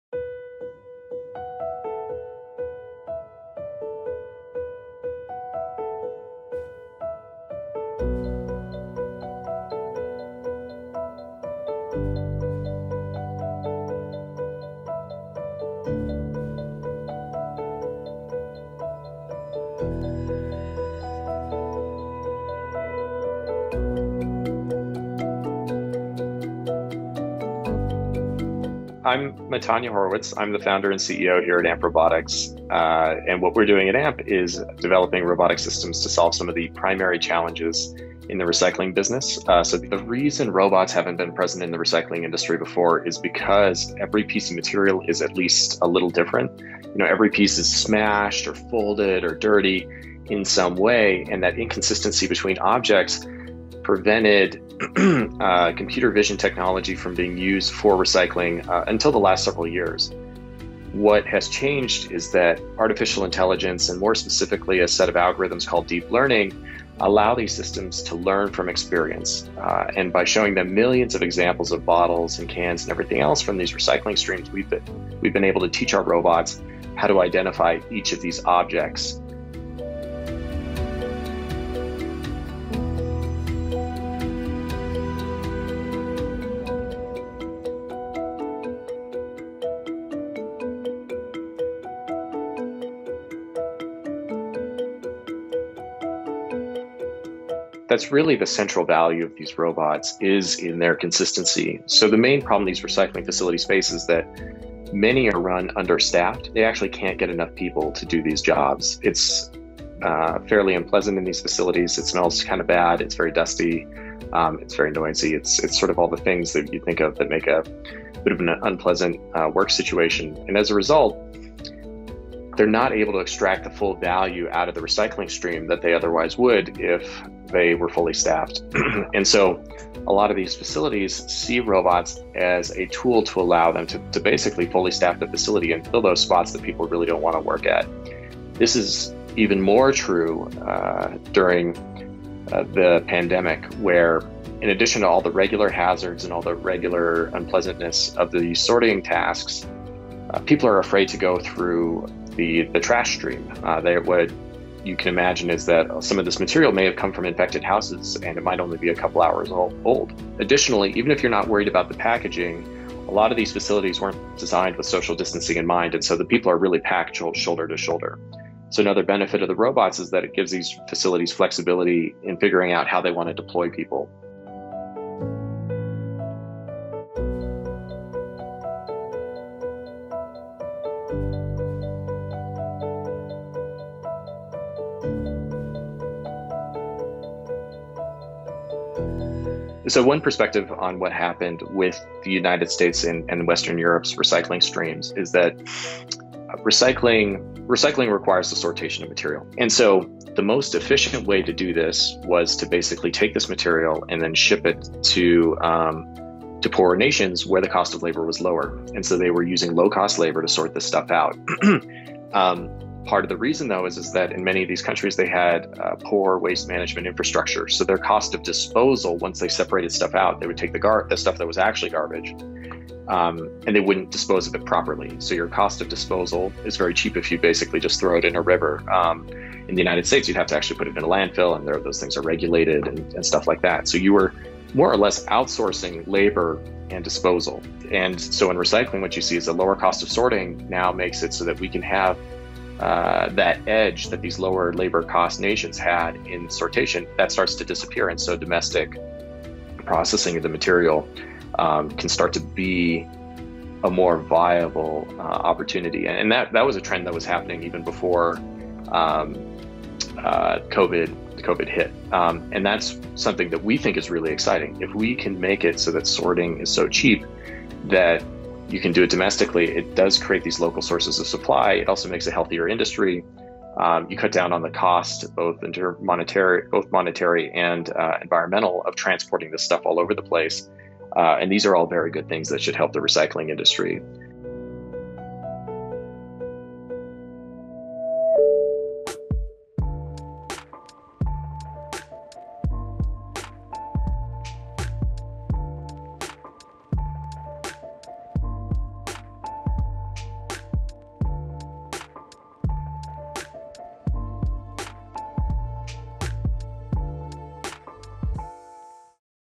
Robots Are Taking Over The Recycling Industry Because Of Pandemic | Mashable
The coronavirus pandemic is transforming the relationship between humans and automation. Robots now serve a dual role – reduce close human contact and keep industries churning.

AMP Robotics, a company that rents out and sells AI-enhanced robots that sort recyclable material, has seen a big boost in orders after the pandemic hit the US.